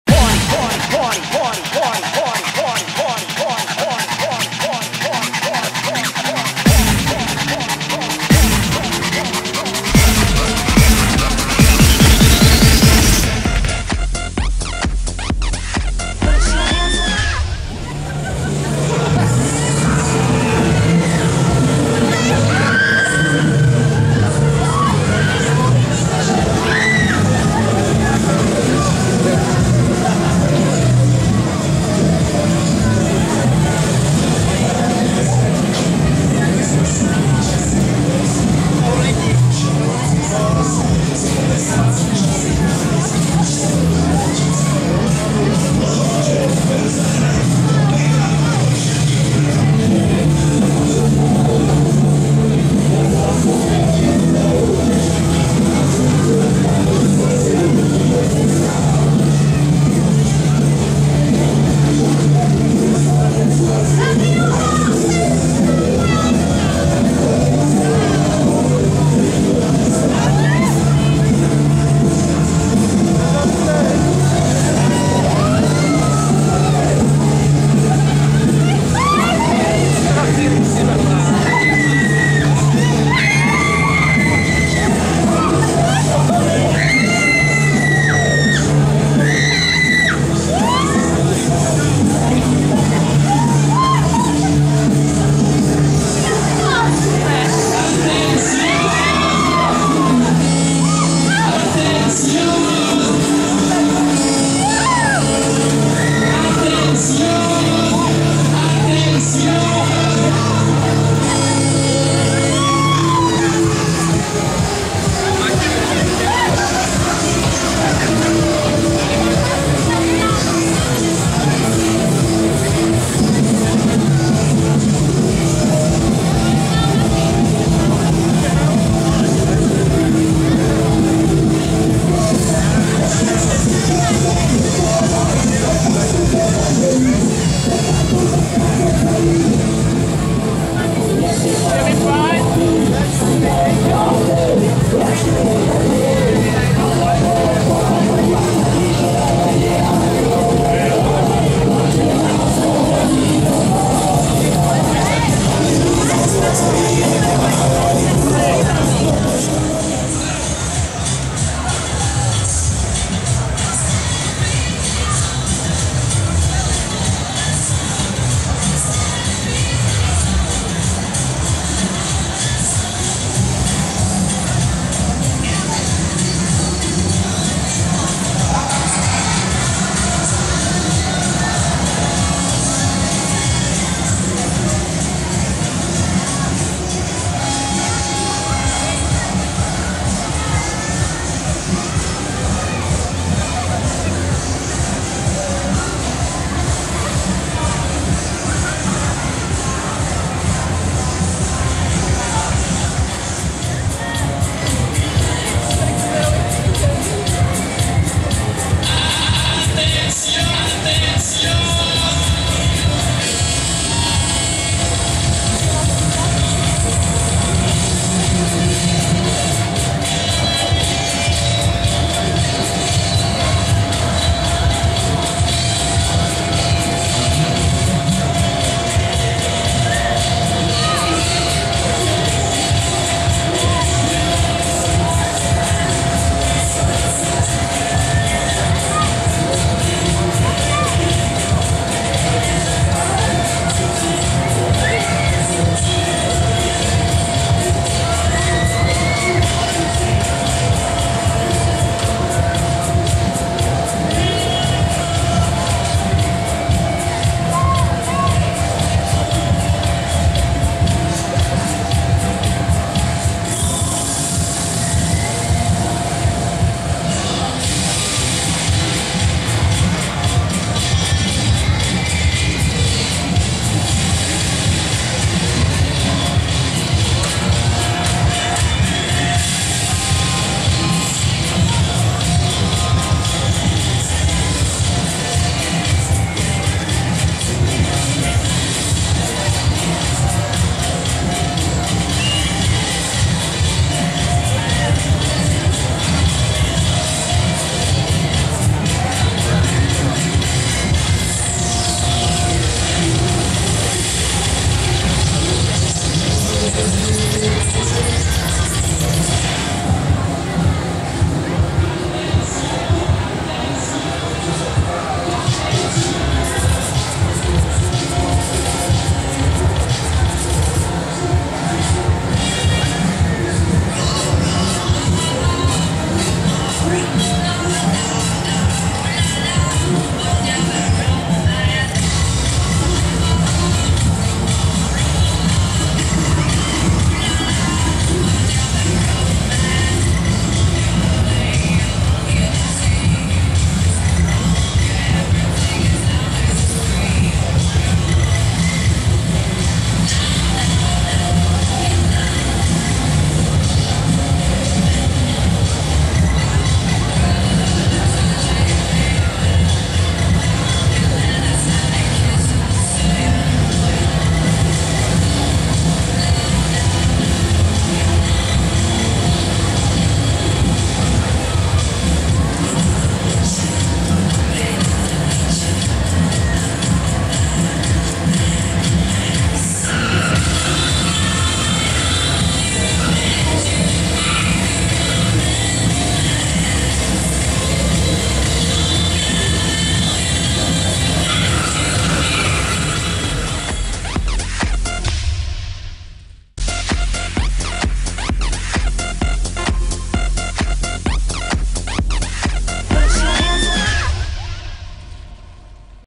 Nymburské posvícení 2012 - ATRAKCE K2 (BOOSTER) / ATTRACTION K2 (BOOSTER) OFF-RIDE
Atrakce K2 (BOOSTER) na pouti v Nymburku v rámci Nymburského posvícení 2012. Jízda pro jednu osobu 150 Kč (150 CZK). Natáčeno v sobotu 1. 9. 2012. Provozovatel Josef Tříška.